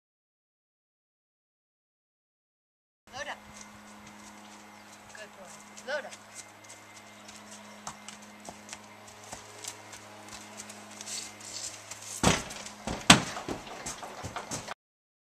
loading fig
The training of Fig (a large mule) to load in a trailer.